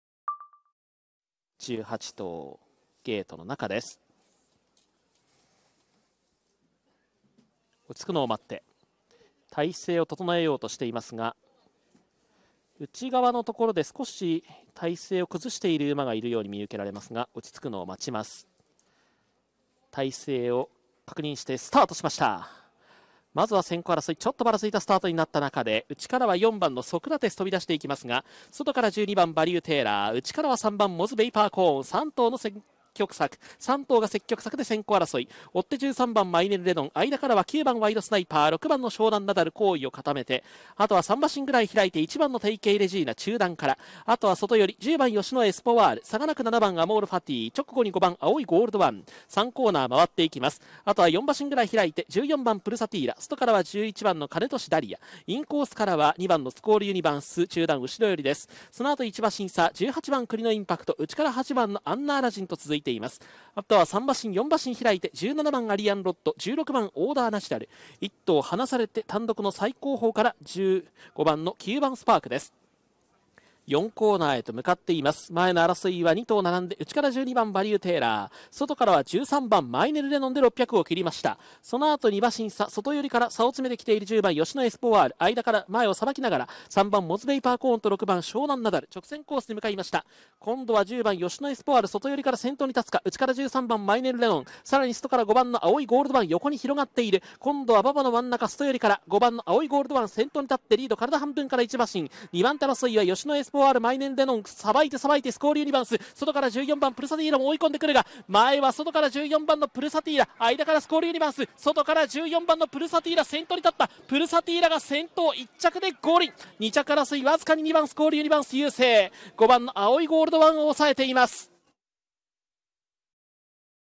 Pulsatilla (JPN) wins race 4 Maiden 1600m (Turf)
2/26/2022 Hanshin Race 4 Maiden 1600m (Turf)
Pulsatilla (pgm#14)
Pulsatilla (JPN)
TB, B, F, Foaled February 21, 2019
Deep Impact (JPN) -  Covert Love (IRE), by Azamour (IRE)
Breeder: Northern Farm